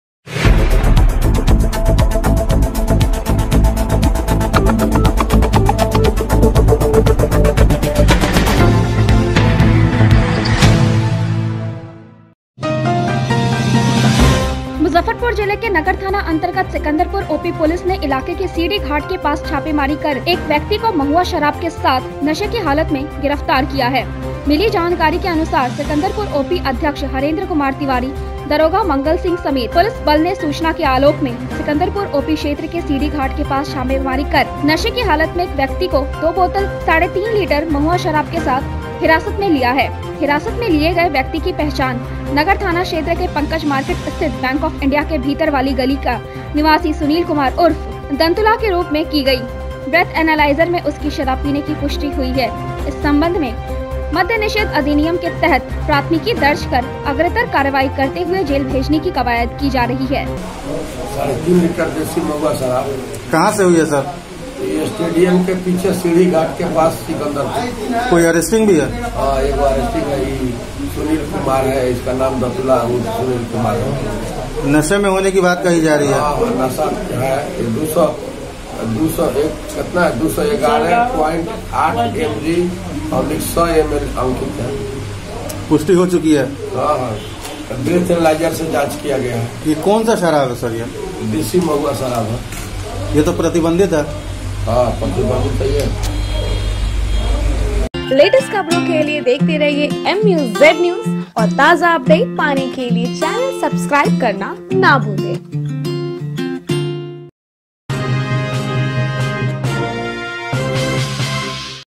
MUZAFFARPUR : सिकंदरपुर में महुआ श'राब के साथ शराबी गिर'फ्तार...
जिले के नगर थानांतर्गत सिकंदरपुर ओपी पुलिस ने इलाके के सीढ़ीघाट के पास छापेमारी कर एक व्यक्ति को महुआ शराब के साथ नशे की हालत में गिरफ्तार किया है.
मिली जानकारी के अनुसार सिकंदर ओपी अध्यक्ष हरेंद्र कुमार तिवारी, दारोगा मंगल सिंह समेत पुलिस बल ने सूचना के आलोक में सिकंदरपुर ओपी क्षेत्र के सीढ़ी घाट के पास छापेमारी कर नशे की हालत में एक व्यक्ति को दो बोतल में बंद 3.5 लीटर महुआ शराब के साथ हिरासत में ले लिया. हिरासत में लिये गए व्यक्ति की पहचान नगर थाना क्षेत्र के पंकज मार्केट स्थित बैंक ऑफ़ इंडिया के भीतर वाली गली का निवासी सुनील कुमार उर्फ़ दंतुला के रूप में की गई है. ब्रेथ एनालाइजर में उसकी शराब पीने की पुष्टि हुई है.
इस सम्बन्ध में मद्य निषेध अधिनियम के तहत प्राथमिकी दर्ज कर अग्रेत्तर कार्रवाई करते हुए जेल भेजने की कवायद की जा रही है.